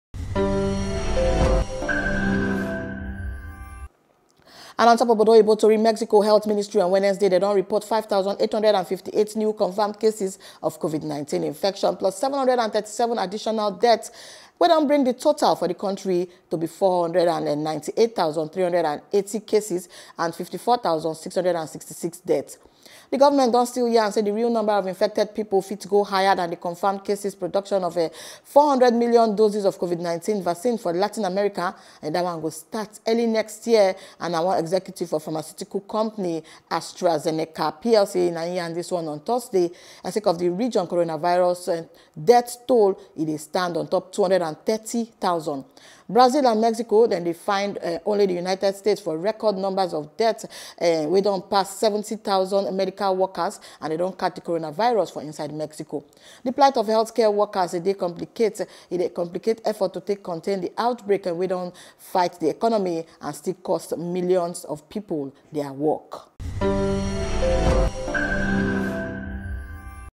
Pidgin News | Covid19 Mata - Mexico Don Confirm Almost 500,000 Coronavirus Cases
Covid19 Mata - Mexico Don Confirm Almost 500,000 Coronavirus Cases